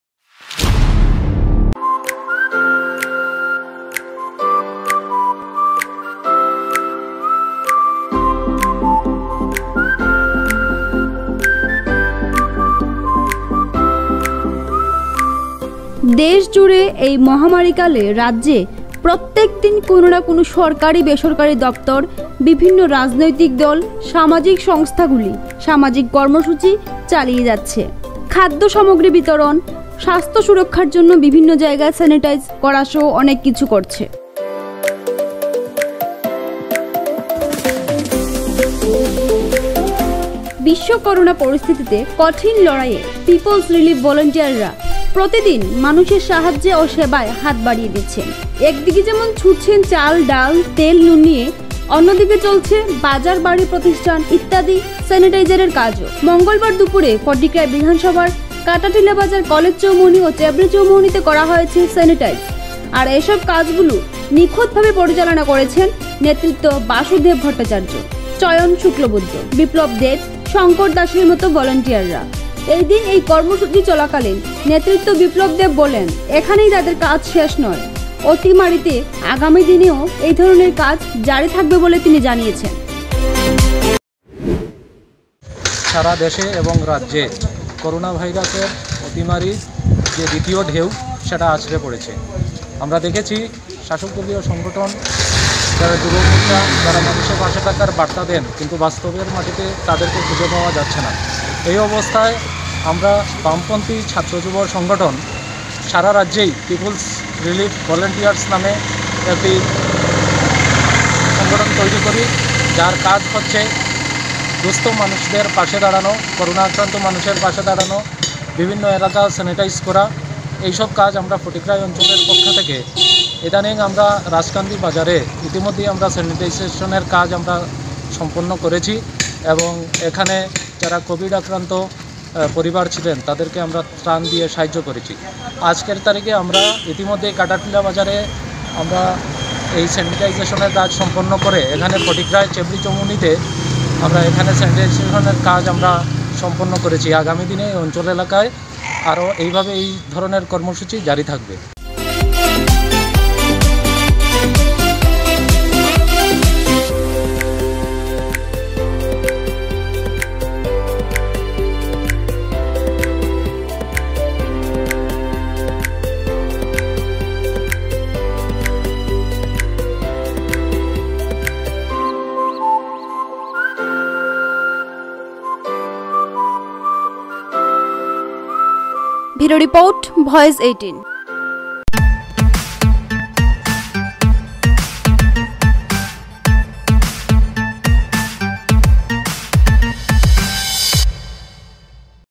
বিশ্ব করোনা পরিস্থিতিতে কঠিন লড়াইয়ে People Relief Volunteers || Kumarghat || VOICE 18
বিশ্ব করোনা পরিস্থিতিতে কঠিন লড়াইয়ে পিপলস রিলিফ ভলান্টিয়াররা। কুমারঘাটেও প্রতিদিন মানুষের সাহায্যে ও সেবার হাত বাড়িয়ে ভলান্টিয়াররা। এক দিকে যেমন ছুটছেন চাল,ডাল,তেল,নুন নিয়ে। অন্যদিকে চলছে বাজার,বাড়ি,প্রতিষ্ঠান ইত্যাদি স্যানিটাইজের কাজও। মঙ্গলবার দুপুরে ফটিকরায় বিধানসভার কাঁটাটিলা বাজার,কলেজ চৌমুহনী ও চেবরি চৌমুহনীতে করা হয়েছে স্যানিটাইজ। আর এসব কাজ গুলো নিখুঁতভাবে পরিচালনা করছেন নেতৃত্ব বাসুদেব ভট্টাচার্য,চয়ন শুক্লবৈদ্য,বিপ্লব দেব,শঙ্কর দাসের মতো ভলান্টিয়াররা। এদিন এই কর্মসূচি চলাকালীন নেতৃত্ব বিপ্লব দেব বলেন,এখানেই আমাদের কাজ শেষ নয়। অতিমারিতে আগামী দিনেও এ ধরনের কাজ জারি থাকবে।
কুমারঘাট থেকে গোপাল মালাকারের রিপোর্ট, VOICE18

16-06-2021

VOICE 18
Name of Production House: FLAXEN MEDIA WORKS